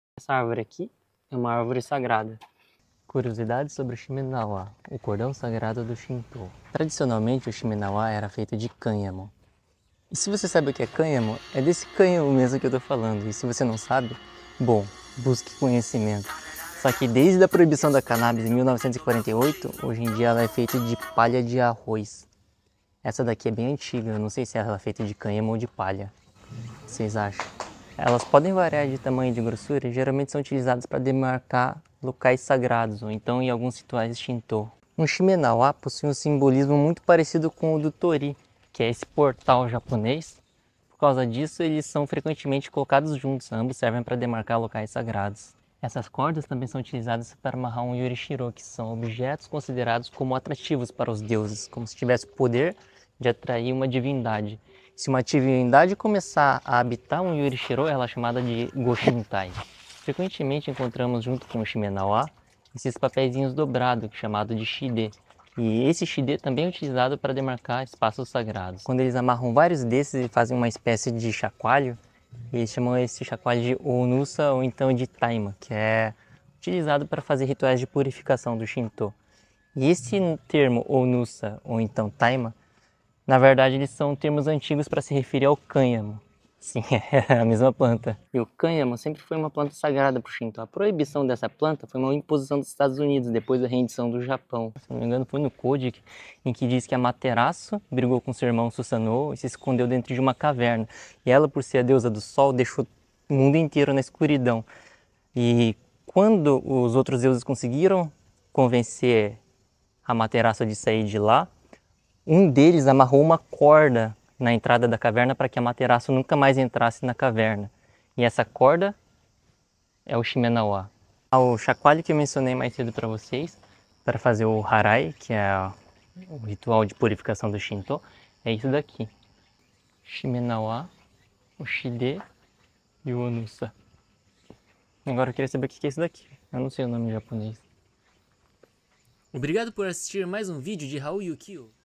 Curiosidades sobre o Shimenawa, o cordão sagrado do Xintoísmo
🌿✨ Shimenawa: A Tradição do Cânhamo no Japão ✨🌿
Você sabia que o shimenawa (しめ縄) era tradicionalmente feito de cânhamo? 🌾 No entanto, após a proibição do cânhamo em 1948, ele passou a ser feito principalmente de palha de arroz. Este cordão sagrado é usado em rituais xintoístas para marcar locais sagrados e afastar espíritos malignos.
Curiosidades:
Alguns lutadores de sumô (相撲) usam o shimenawa em suas cerimônias de purificação antes das lutas! 🥋 O yokozuna (横綱), o mais alto título no sumô, usa um shimenawa ao redor da cintura durante cerimônias importantes, simbolizando sua pureza e força espiritual. Ele é considerado um yorishiro vivo, um receptáculo temporário para os kami, e em algumas tradições, um goshintai.
No mito da caverna, a deusa do sol Amaterasu (天照大神) se escondeu em uma caverna após uma briga com seu irmão Susanoo (須佐之男命). Após ser persuadida a sair, a caverna foi selada com um shimenawa para evitar que ela voltasse a se esconder.
O ritual de harae (祓え) representa a purificação e a expulsão de espíritos, e simboliza o momento em que Susanoo foi expulso do mundo celestial.
Descubra mais sobre essa fascinante tradição e sua importância na cultura japonesa!
*Localização: Kanmuri Inari Jinja
*Gunma ken
*Ota shi
Você já viu um shimenawa em algum lugar que não seja em um santuário xintoísta?